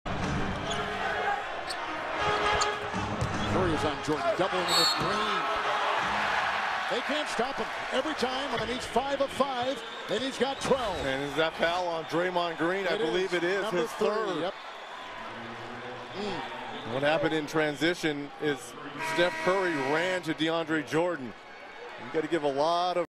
NBA, playoff 2014, Clippers vs. Warriors, Round 1, Game 5, Move 22, DeAndre Jordan, 2 pointer
Los Angeles Clippers - Golden State Warriors, Game 5, 29/04/2014, quarter 2
DeAndre Jordan, 2 pointer